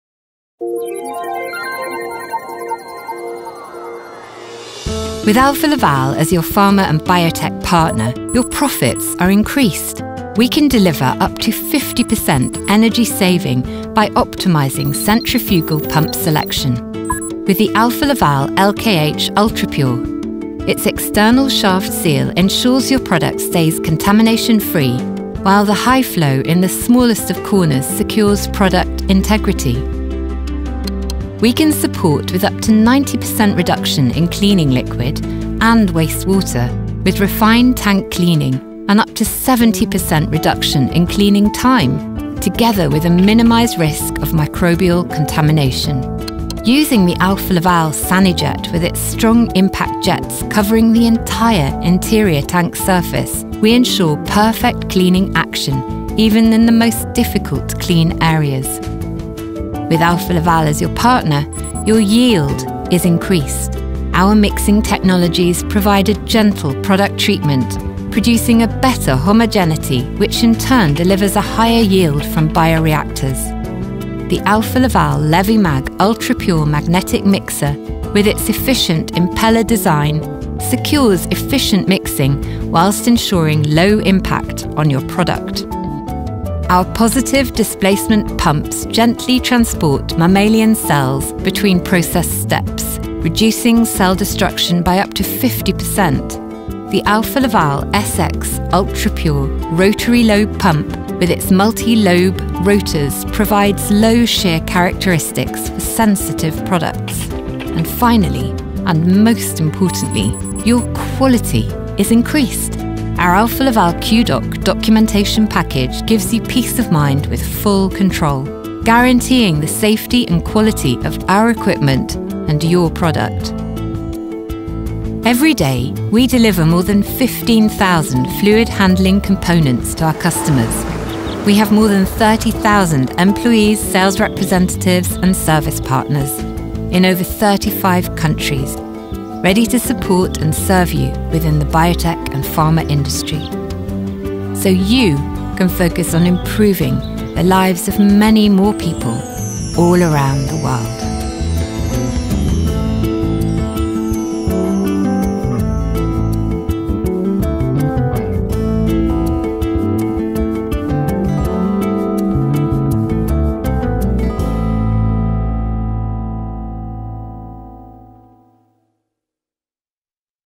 Alfa Laval is Pharma & Biotech - Achieve more with our Fluid handling equipment
How does our fluid handling equipment help you to reduce liquid consumption, increase yield, and maintain product quality? Watch to learn more about the benefits our pumps, tank cleaning and mixing equipment can bring to your pharma and biotech process lines.